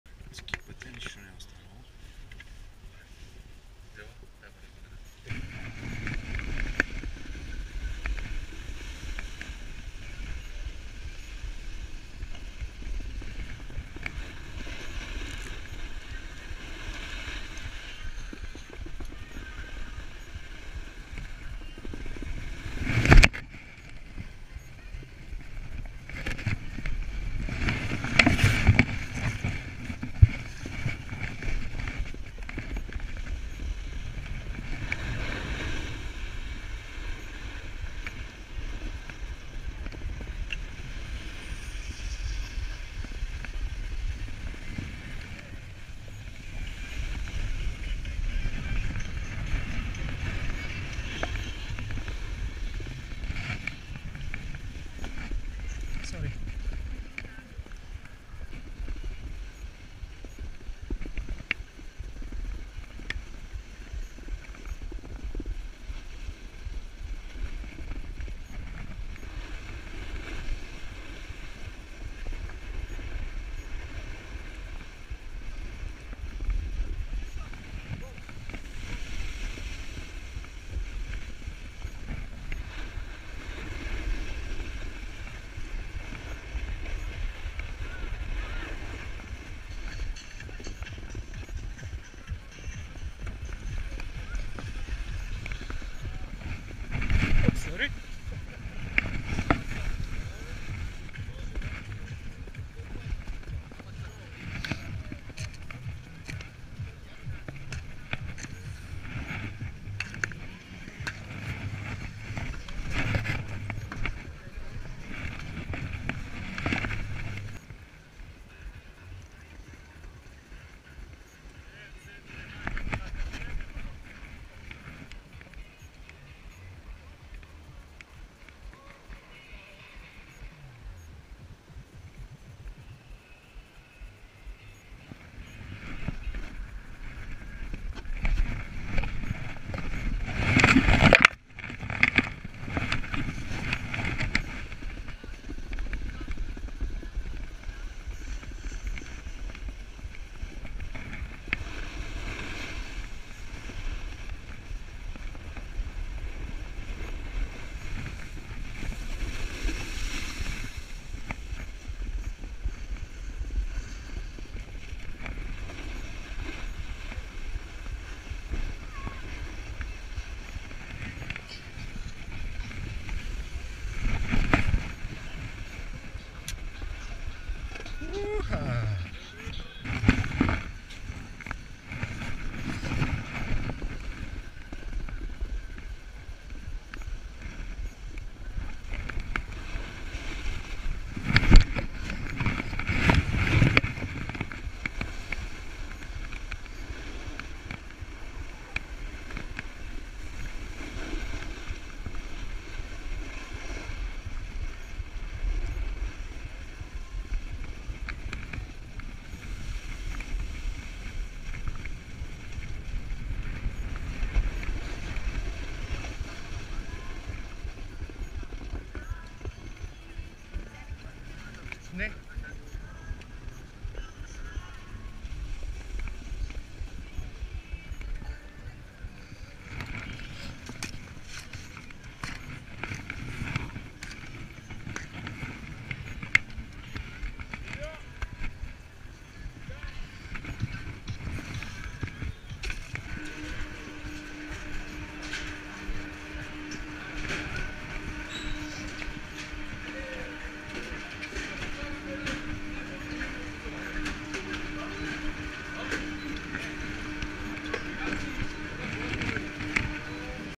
Bansko 2016
First steps in snowboarding, Bansko, Bulgaria, March 2016!